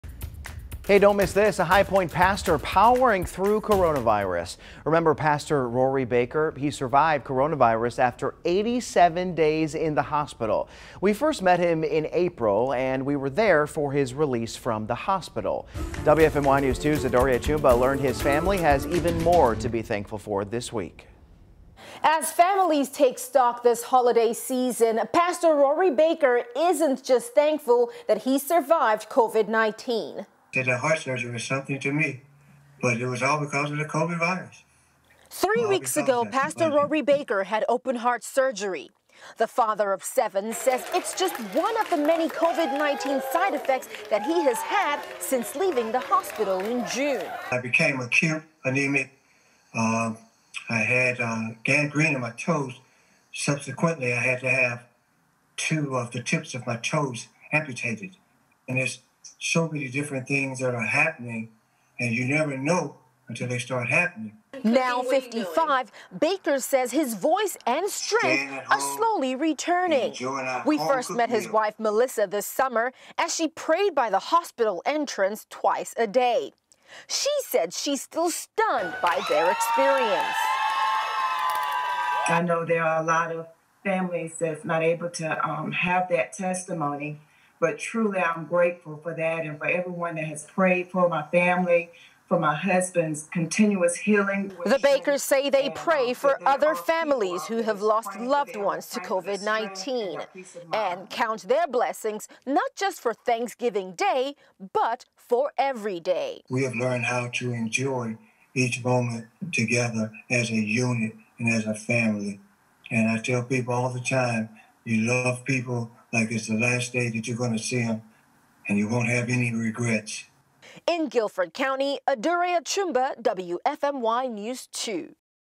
High Point Pastor Rory Baker survives COVID-19, has more to be thankful for
Pastor Rory Baker had open-heart surgery as a result of COVID-19 complications. He says he prays that families can heal if they lost a loved one to coronavirus.

Before he came down with COVID-19 in April, Baker led the congregation at Fruit of the Spirit Ministries in Greensboro. He spent 87 days at @AtriumHealthWakeForestBaptist High Point Medical Center.

"I became acute anemic, I had a gangrene on my toes so subsequently I had to have two of the tips of my toes amputated and there are so many different things that are happening and you never know until they start happening," Baker said.

Now 55, after celebrating a birthday in September, Baker said his voice and strength are slowly returning. Full story

'To God be the Glory' | Triad Pastor leaves hospital after winning life-threatening battle against COVID-19

She prayed by her husband's hospital window every day as he battled COVID-19. Then, someone unexpected prayed beside her.